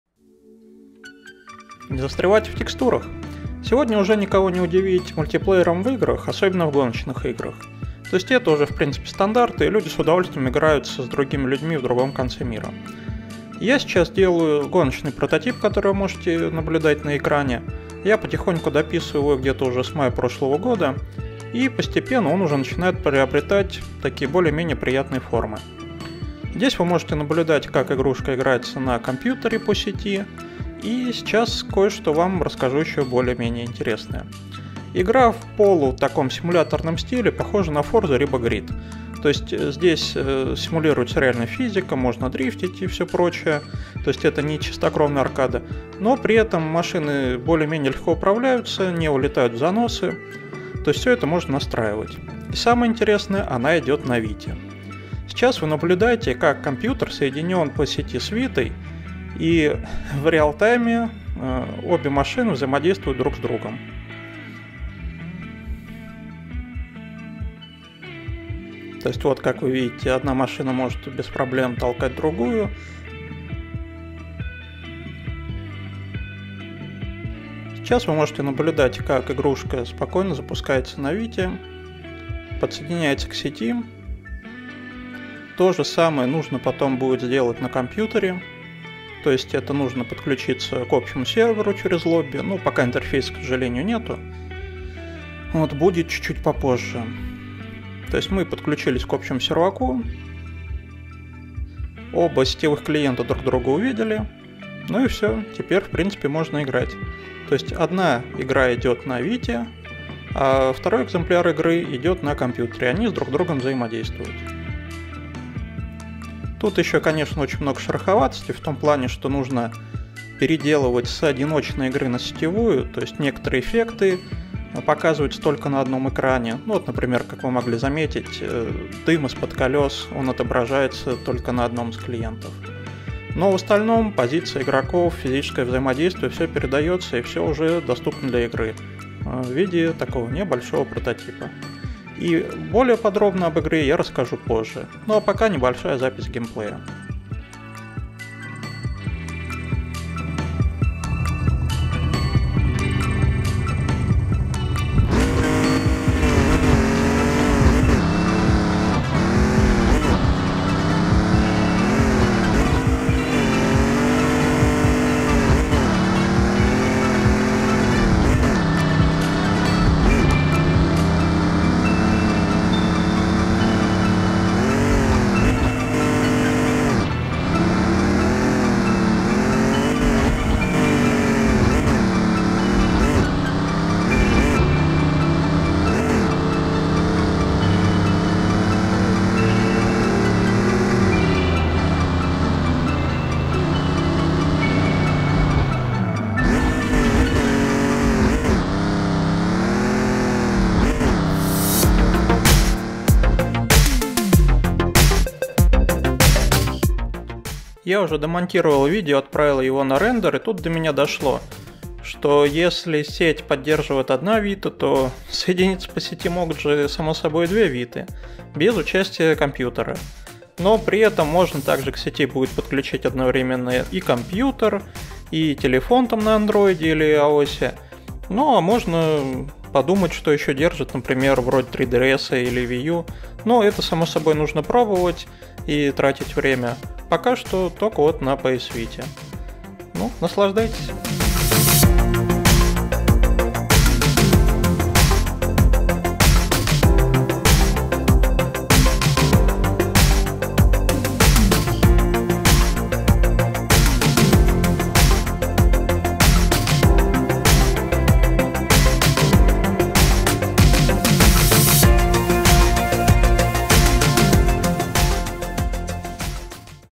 Мои гонки для Ps vita и Pc работают по сети
В свободное от работы время я пишу гоночный прототип на unity и постепенно все складывается в рабочий проект. Сам прототип я тестирую для PC и более слабой ps vita, что накладывает определенные ограничения. Но открываются и новые возможности - кроссплей разных платформ. И в видео я показываю как это все работает на нынешний момент.

Другие небольшие видео по разработке этой гоночной игры.

Обращаю внимание, что это довольно сырая версия, я на ней тестирую возможности игры и какие ограничения у платформы. Одиночная игра (build04_DriftTeacherTrail.vpk) идет в 50-60 фпс, я ее давно тестирую и скоро буду добавлять меню, кастомизацию, авто, итд. Сетевая же (build05_network.vpk) собрана сегодня с утра и я ее не тестировал. Точно могу сказать, что если в лобби будет больше 20 человек, то больше в него никто не зайдет. И, думаю, что больше 5 человек на серваке уже сильно нагрузят физическую систему, но, опять же, не проверял еще. А, ну и ставится это все на взломанную систему. Если кто знает, как можно хоумбрю запускать на невзломанной системе - пишите, тема интересует.